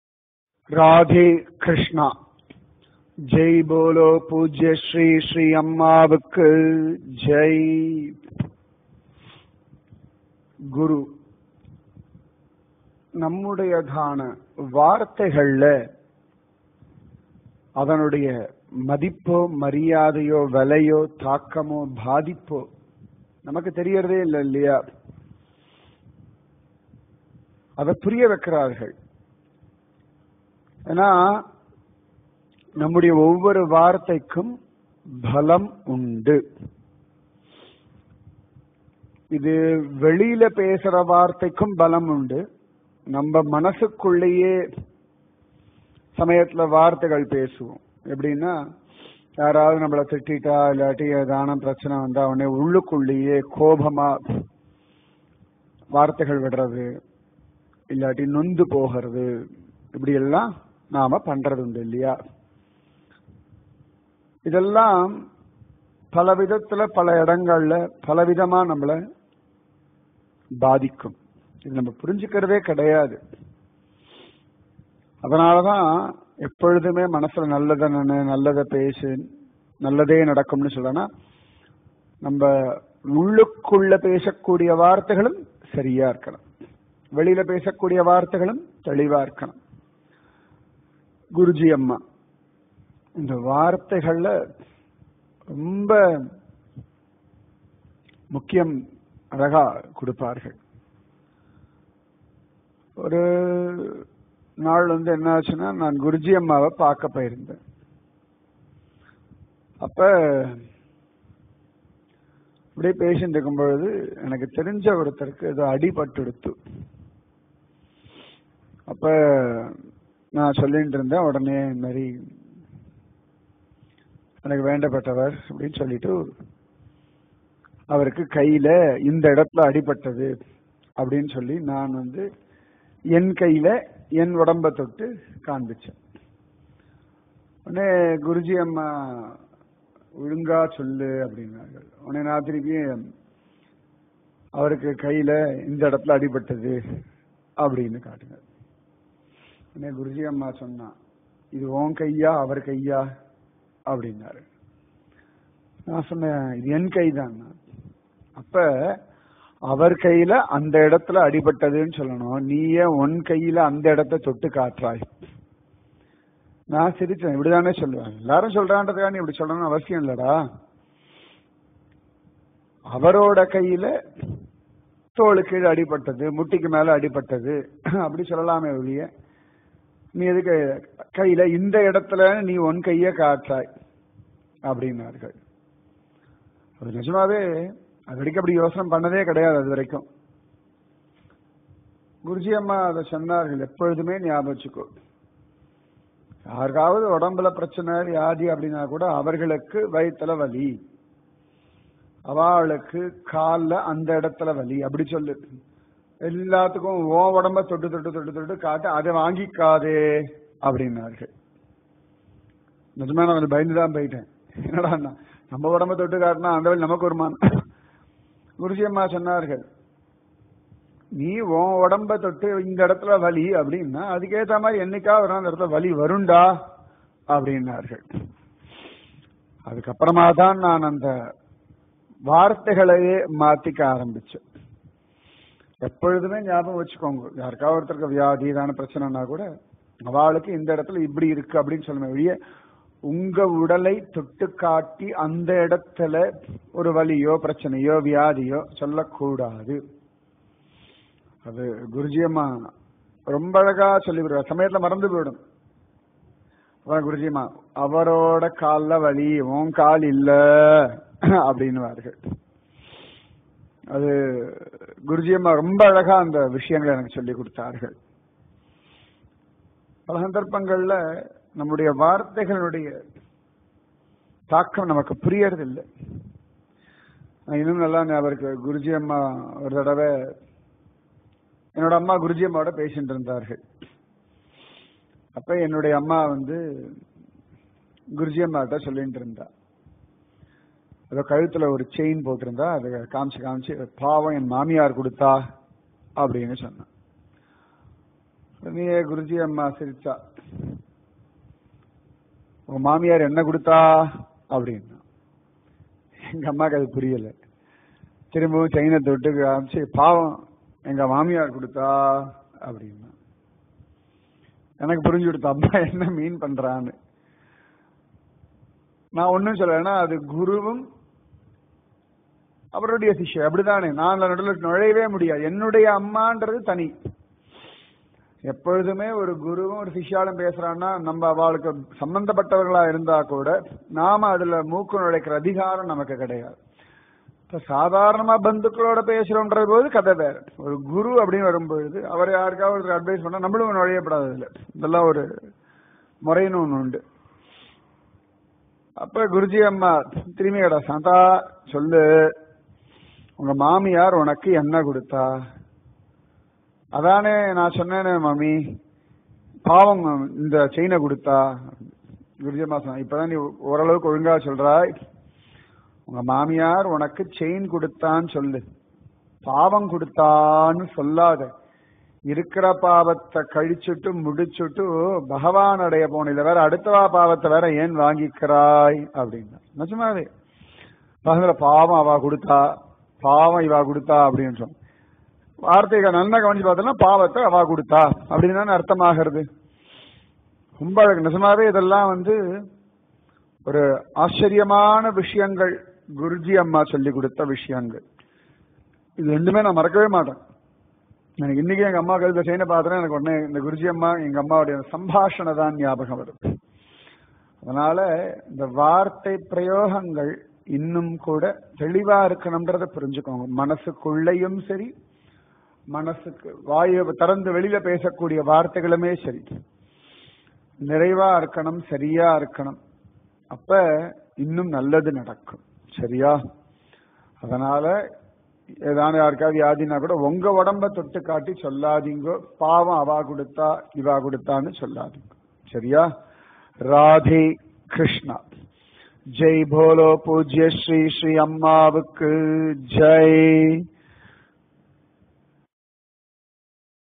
65 | Me and my Guru | Words
Guruji Gopalavallidasar shares his experience and interactions with his Guru Poojya Shree Shree Amma. Situations from everyday life teach invaluable lessons.

This is part of the series “Me and Guru”, originally recorded in 2017.

Apara Mahima Guru Mahima